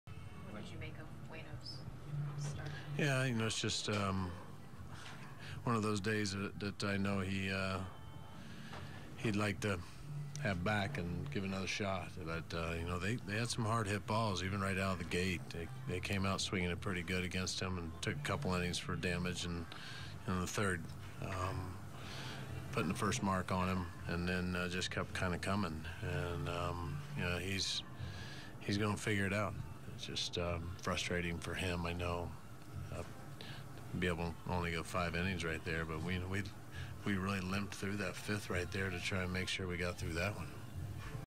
Mike Matheny on Cardinals’ 12-10 win over Angels
Mike Matheny talks about a game in which the Cardinals overcame a rough start by Adam Wainwright and a tight ninth-inning situation to hold on for a sweep over the Angels.